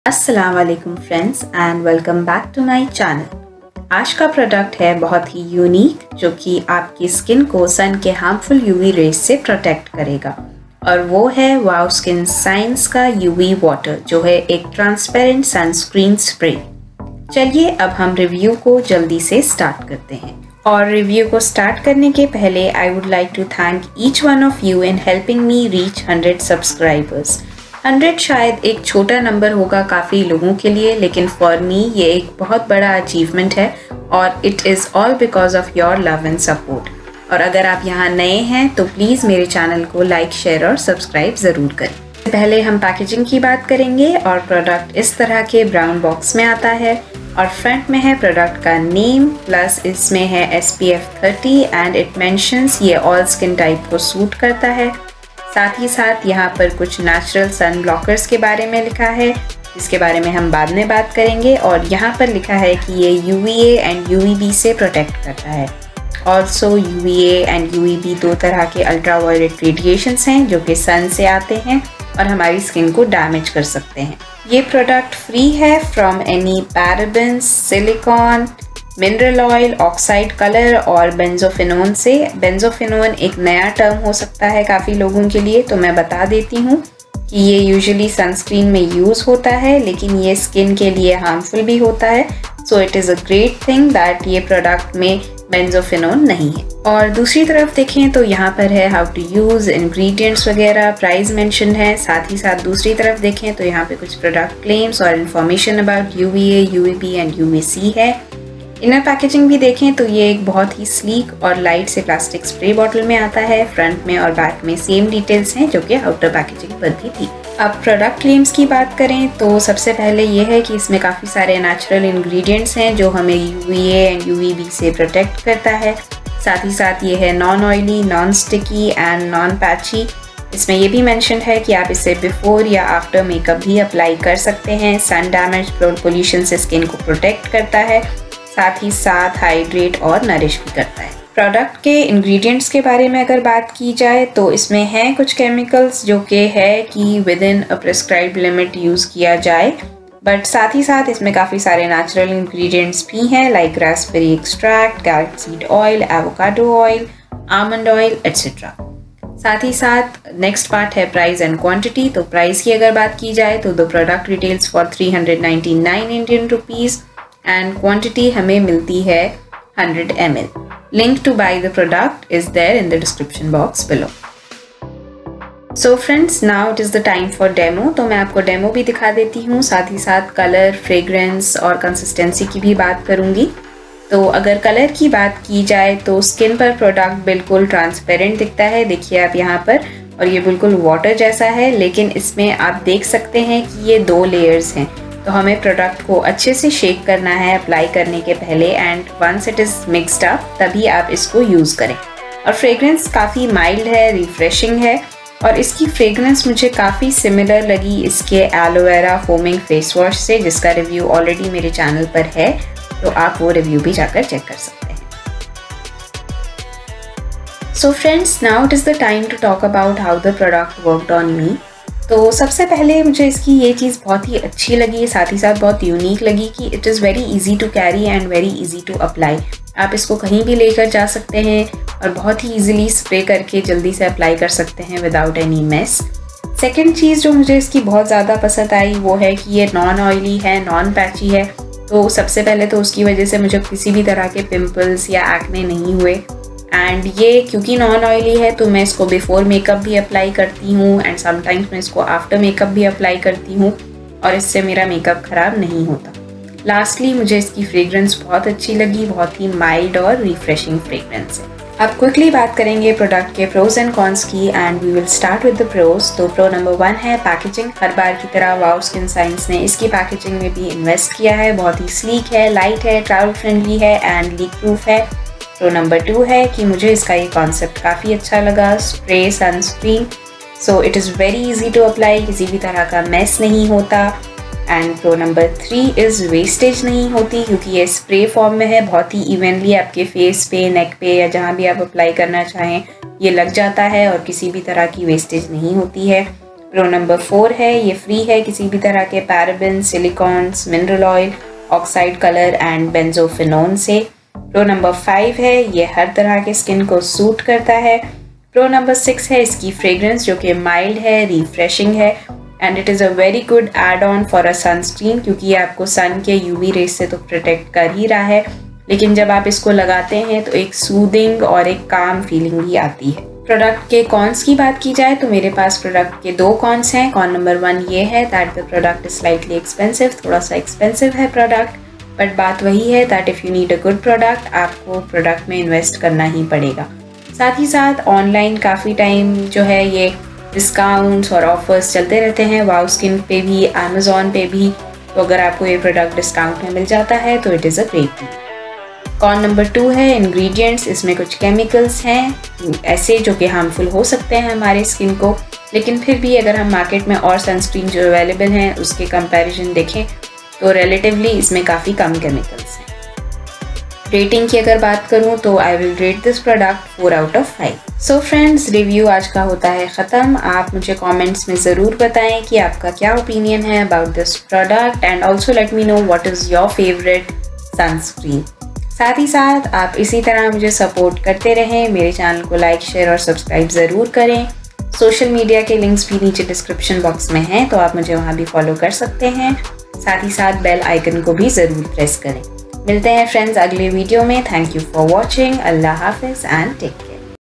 *NEW* WOW Skin Science UV Water|Transparent Sunscreen Spray SPF 30| Honest Review
WOW Skin Science UV Water Transparent Sunscreen Spray SPF 30 is a unique, natural, sunscreen product that is quite new to the Indian market. Here I present my take on this sunscreen spray-

Benefits
1. Keep your skin hydrated and protected from sun’s harsh rays with WOW Skin Science Sunscreen Water Spray SPF30++.
2. It gives your skin effective broad spectrum protection against sun’s harmful UVA & UVB rays.
3. Its non-oily, non-sticky and non-patchy formulation spreads instantly and evenly to create a transparent film that shields your skin from sun damage and pollutants.
4. No streaks, no mess, no fuss. Just spray as and when required.
5. Suitable for all skin types